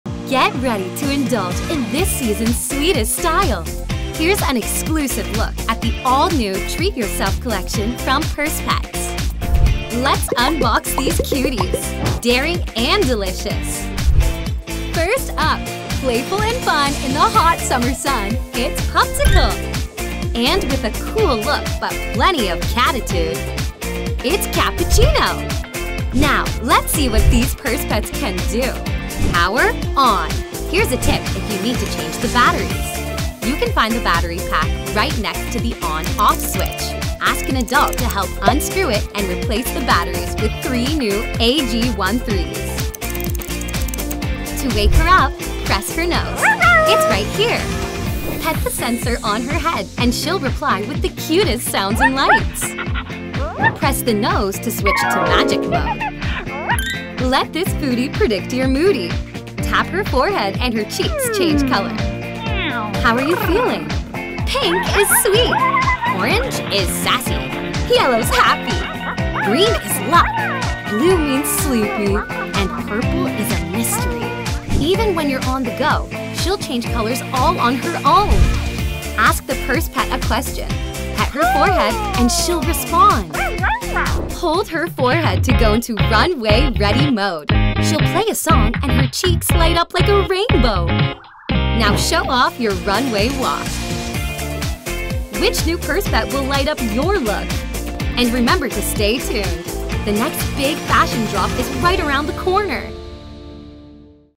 How to Use Treat Yo Self Purse Pets 🍦💙 | Toys for Kids
Treat Yo Self to the all-new collection from Purse Pets! They’re ice cream themed purses that really light up! Here’s everything you need to know to get started with your Purse Pet!

Meet your new Fluffy Fashion BFFs! These fluffy and fabulous interactive Purse Pets (perfect gifts for girls!) are packed with purse-onality.